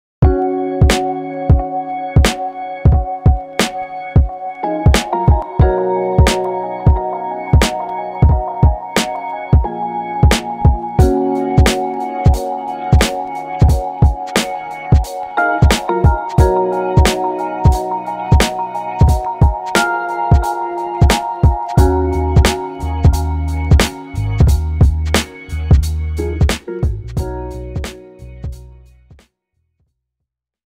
Wisconsin River Drone
First flight with my drone, shot on the Wisconsin River, near Wisconsin Dells, Wi.